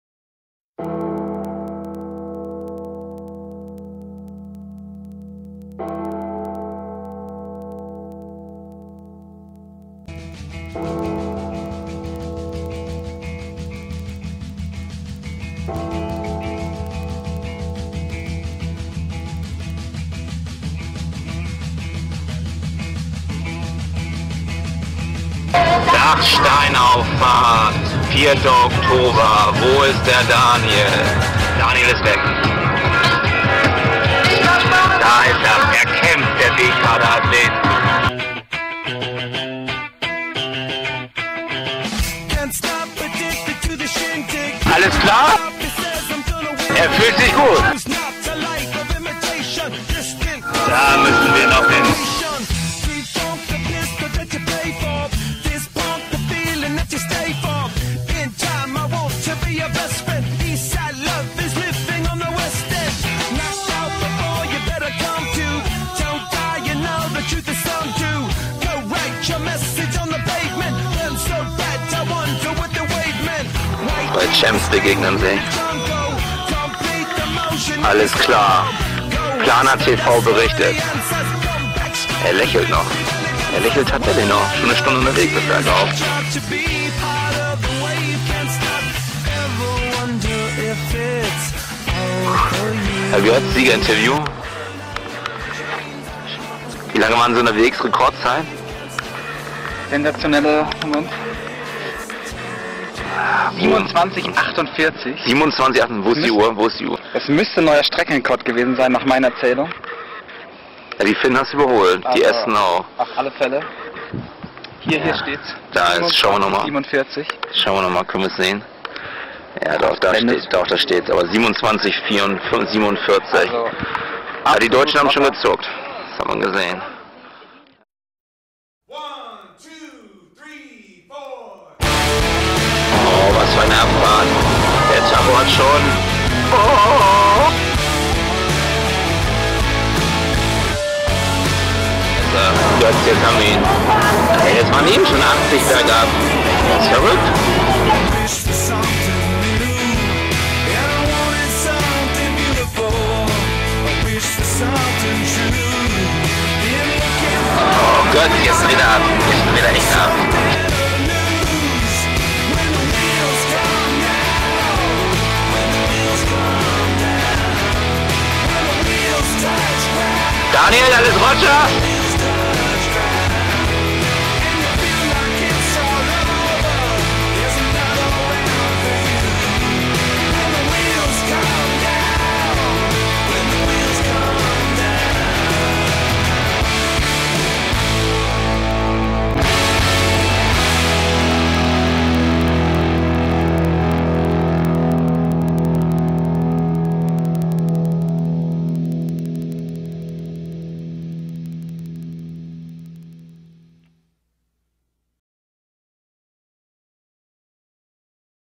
Wo ist Daniel - Der Film
Trainingslager Ramsau / Dachstein - Bayerische Hindernisläufer machen Druck ! Rekordaufstieg zum Dachstein.